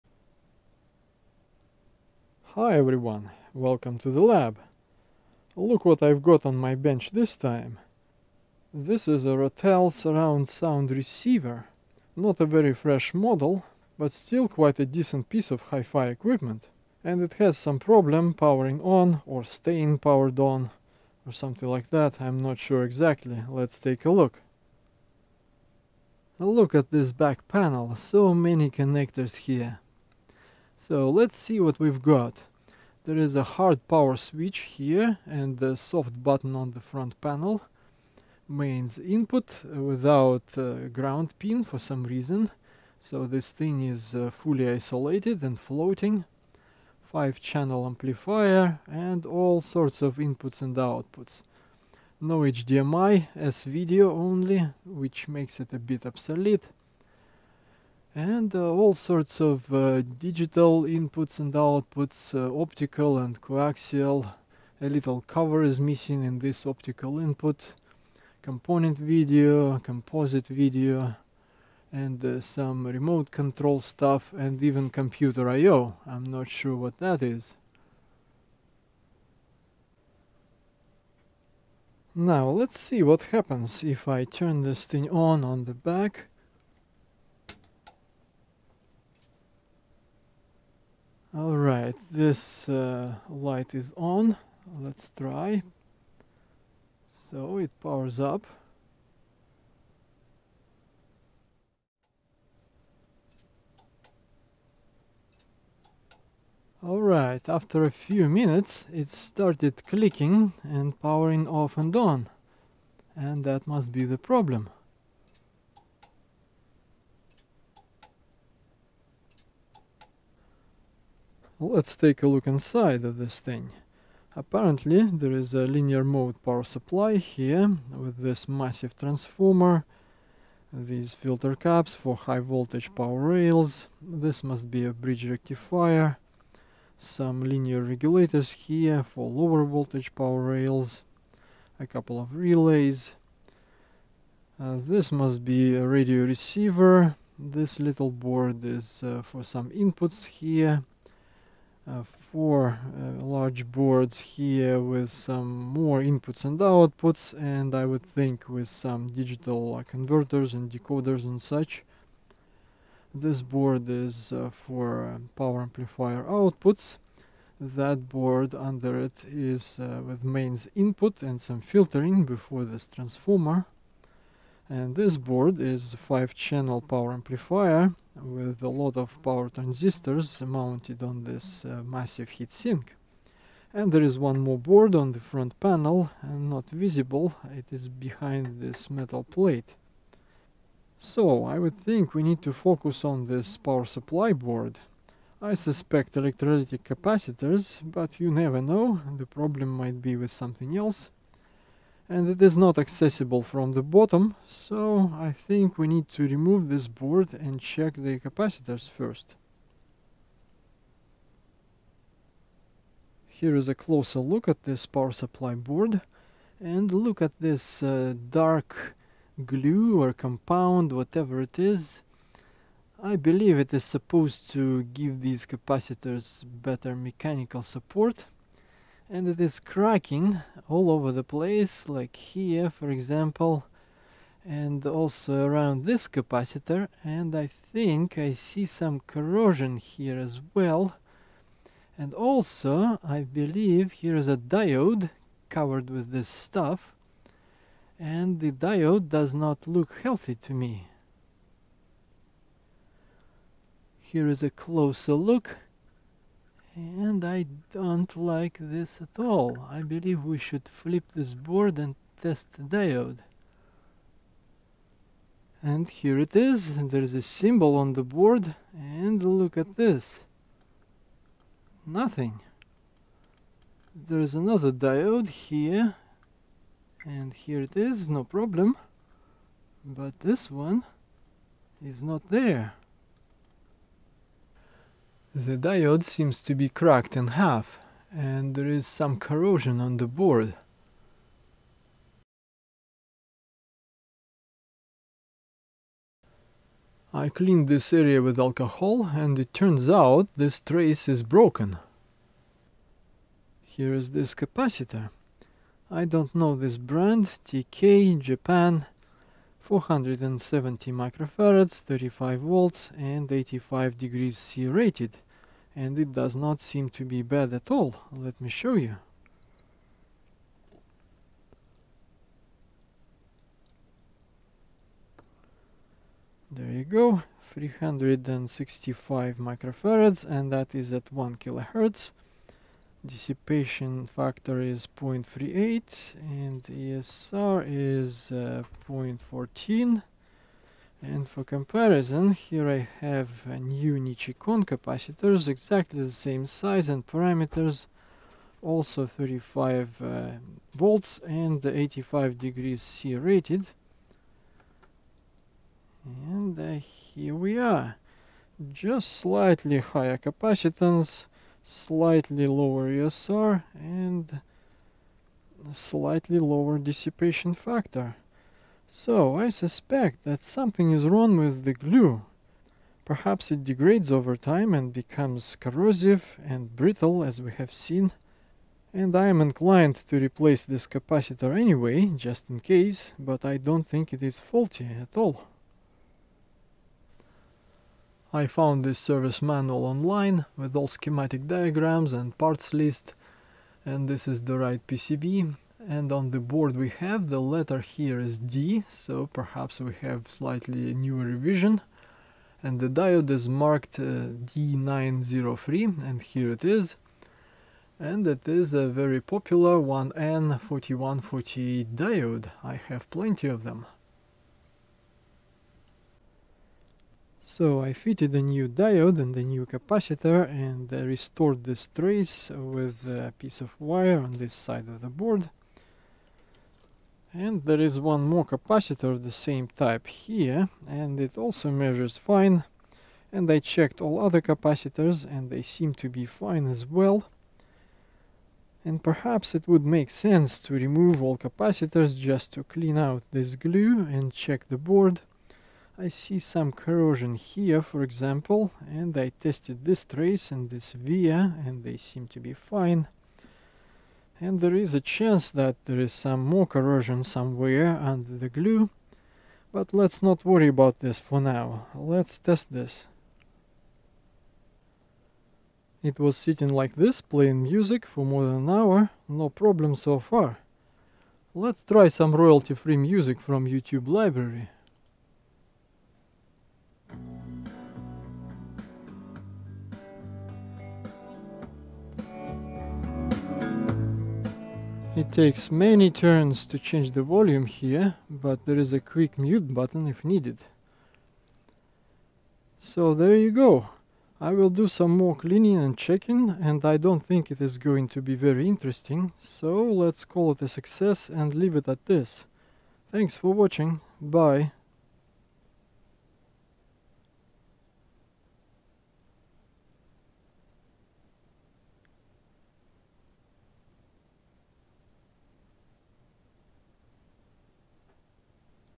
#103 - Rotel RSX-1056 repair
This receiver did not stay powered on, but clicked and went off and on repeatedly. The problem seemed to be with some sort of a glue, which was supposed to hold large capacitors in place, but it turned brittle and corrosive. So a diode was damaged and a PCB trace was eaten away by this corrosion. After replacing the diode, restoring the trace, and replacing a capacitor (not a bad one, but just in case), the receiver came back to life.